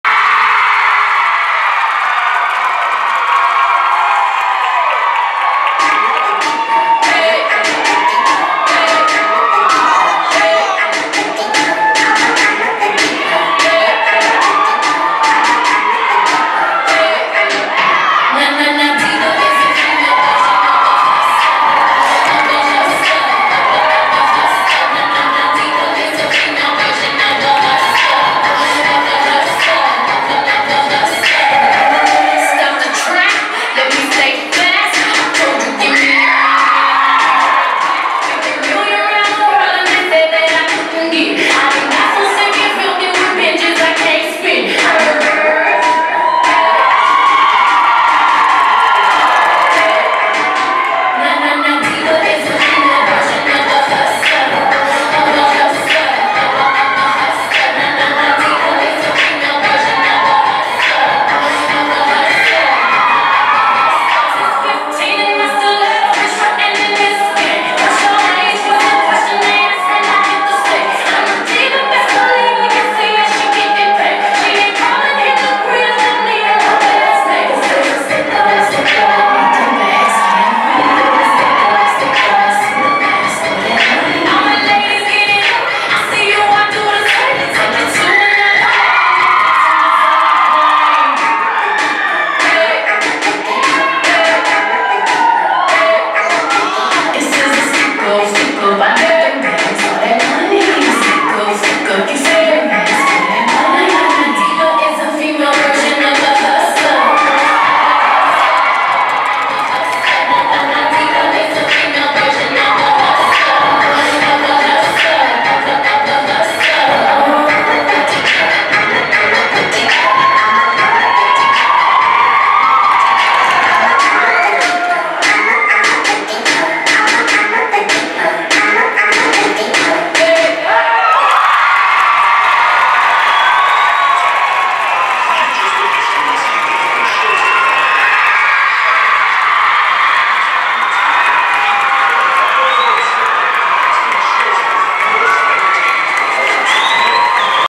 Diva
DeCamp's Spotlight School of Dance
Red Company Jazz
Choreography:  Lindsay DeCamp Fullwood
Precision Award, PAC Attack Challenge Finalist
Detroit 2019